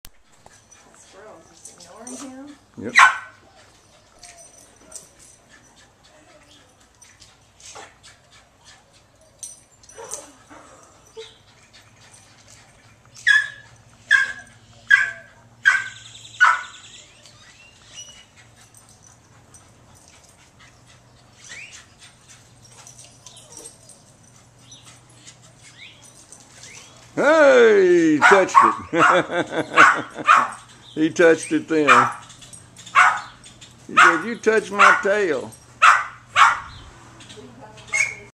Bean gets squirrel.MP4
Watch the close up tail bite toward end of video..
Our Rat Terrier / Boston Bull mix (Bean) has been after this squirrel for 6 months.
Tree bounce jump.